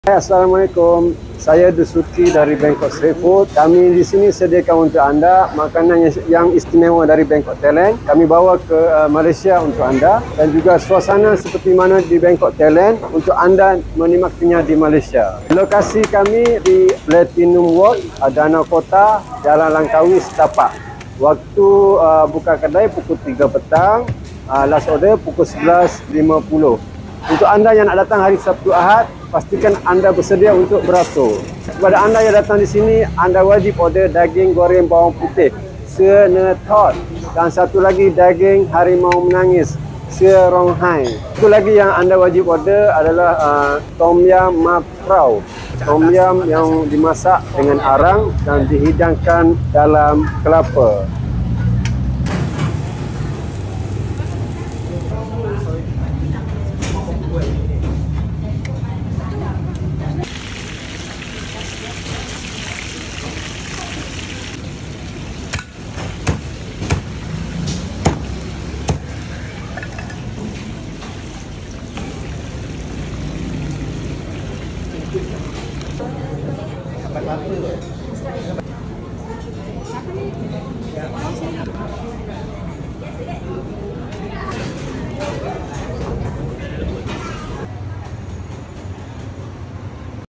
10 jenis Makanan Tepi Jalan dari Bangkok yang jarang jumpa kat Kuala Lumpur.
Memang rase macam makan street food kat Bangkok.

📍Banngkok Street Food
Platinum Walk, 53100 Kuala Lumpur.
Basement Parking Available
⏰Setiap Hari :
3pm - 12am